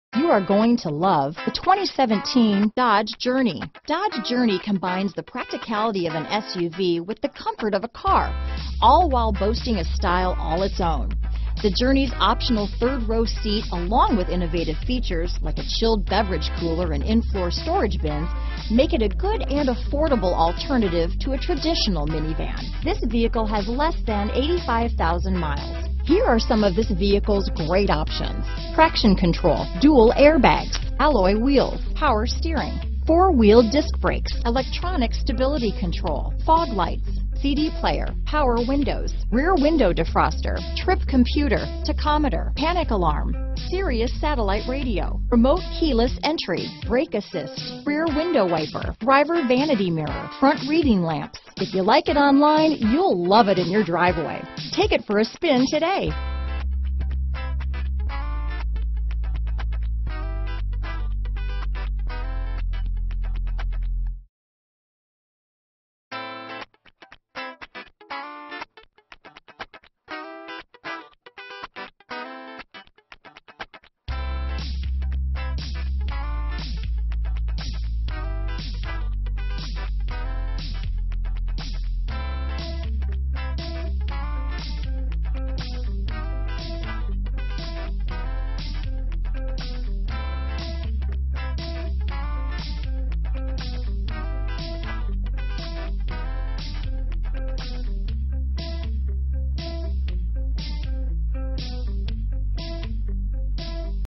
2017 Dodge Journey Willoughby, Painesville, Chardon, Chesterlane, Mayfield Heights OH P1242
Used 2017 Dodge Journey available in Willoughby, Ohio at Chrysler Dodge Jeep Ram of Willoughby. Servicing the Painesville, Chardon, Chesterlane, Mayfield Heights, OH area.

Recent Arrival! Journey SXT 4D Sport Utility 3.6L V6 24V VVT 6-Speed Automatic FWD Pitch Black Clearcoat black Cloth Quick Order Package 28E.